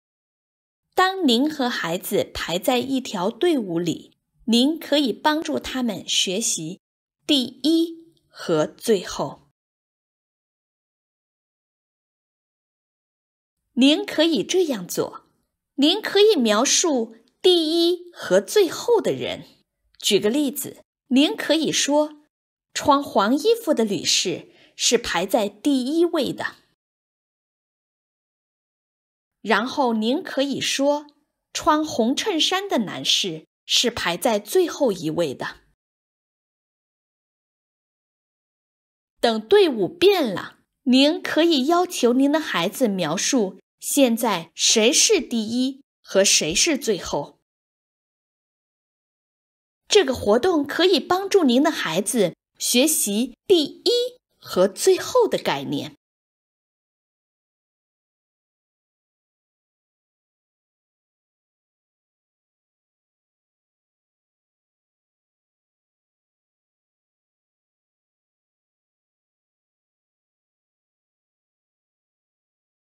第一和最后 - First and Last (Chinese)
在排队的时候，你可以帮助孩子学习第一和最后。这个活动也可以练习使用描述的字。

Chinese translation by Ming Liu, and Xueni Peng. Narration by Xueni Peng.

You can help a child learn about first and last while standing in line. This activity also helps practice the use of describing words.